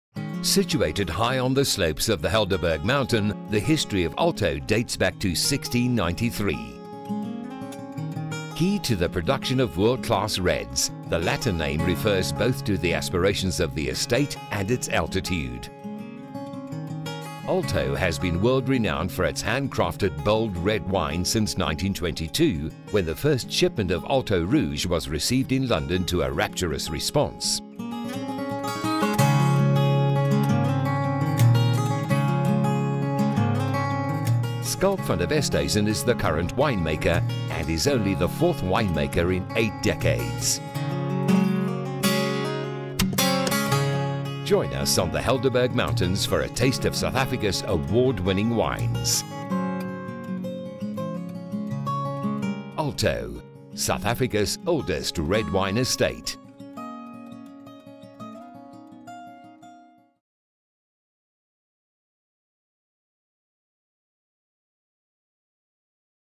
Alto Wine Estate Stellenbosch South Africa - Africa Travel Channel
Situated high on the slopes of the Helderberg mountain, the history of Alto dates back to 1693. Key to the production of world-class reds the Latin name refers both to the aspirations of the estate and its altitude. Alto has been world renowned for its hand crafted, bold red wines since 1922 when the first shipment of Alto Rouge was received in London to a rapturous response. Schalk van der Westhuizen is the current winemaker, and is only the fourth winemaker in eight decades. Join us on the Helderberg Mountains for a taste of South Africa's award winning wines. Alto. South Africa's oldest red wine estate.